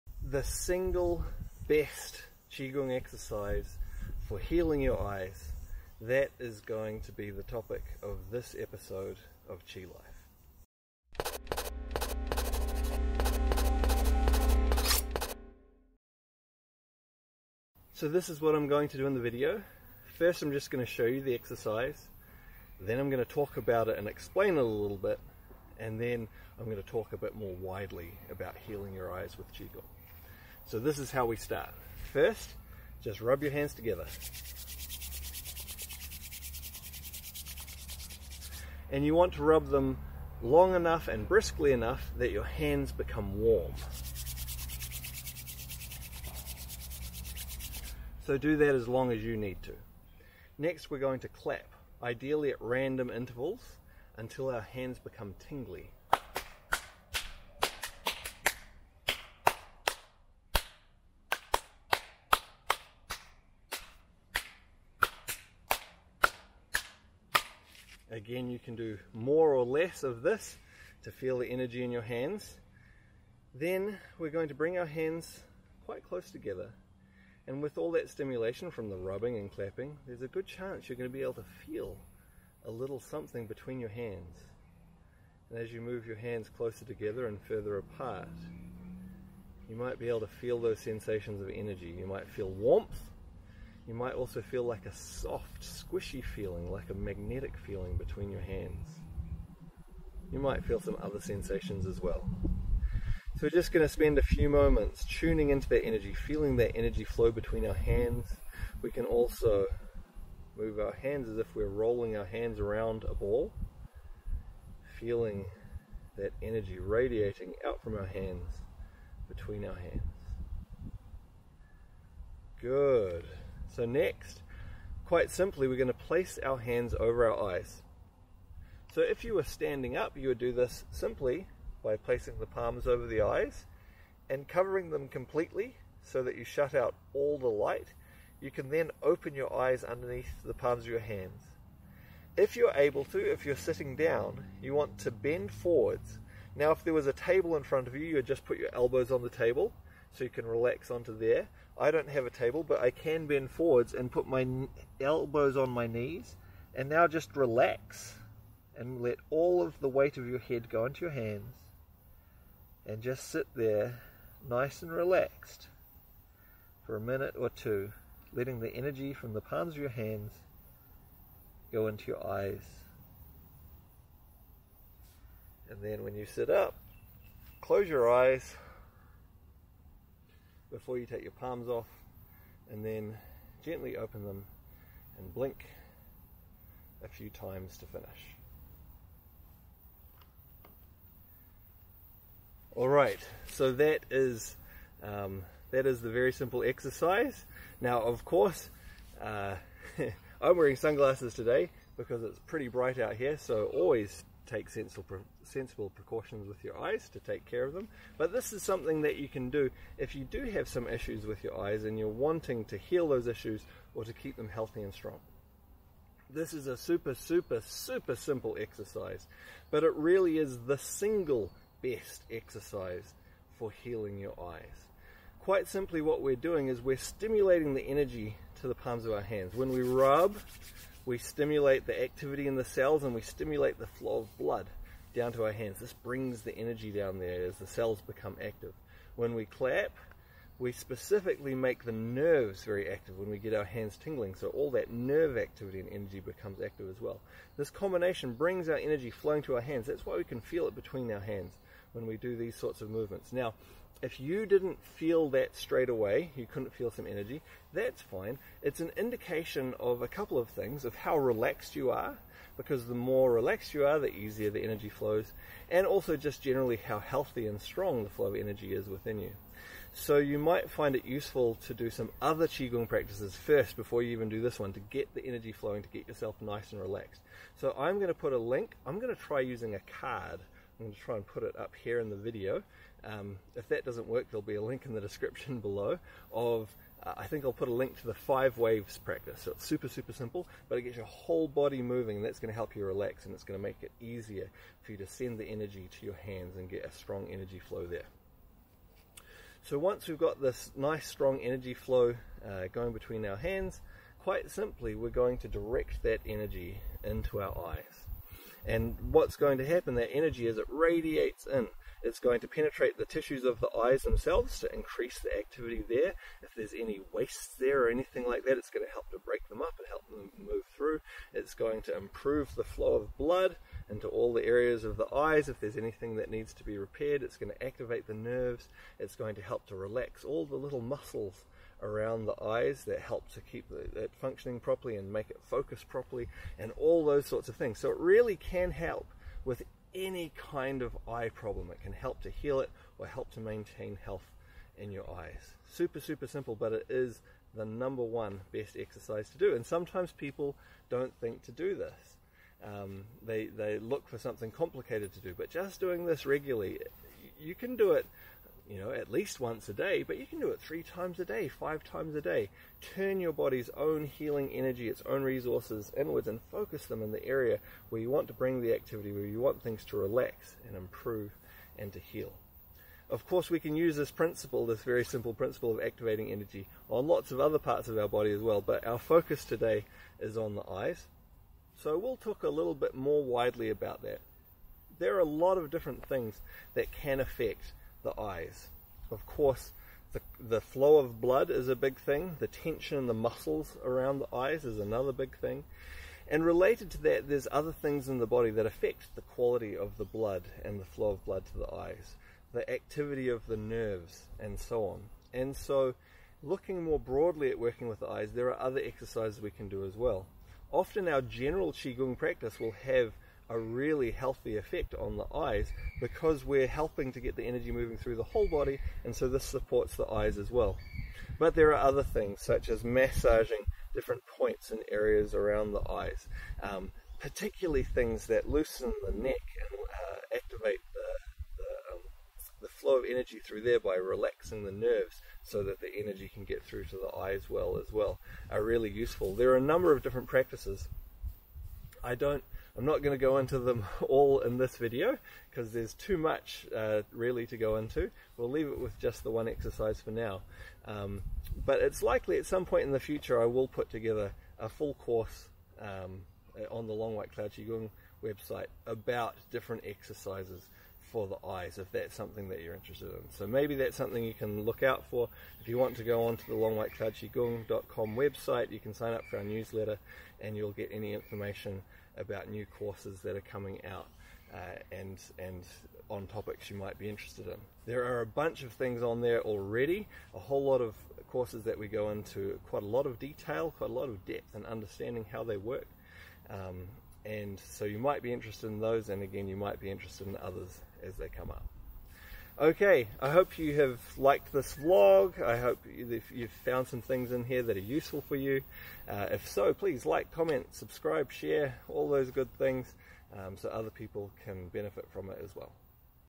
The Single Best Qigong Exercise For Healing Your Eyes - Qi Life - Vlog 67
In this episode I talk about the super simple exercise for the eyes, and all the benefits that you can get from doing it regularly.

Timecodes:

00:00 Showing the exercise
03:12 Explanation of the exercise
04:30 Get the energy flowing before the Eyes Exercise
05:00 Benefits of the exercise
06:13 The things that affect the eyes
06:46 Practices for the eyes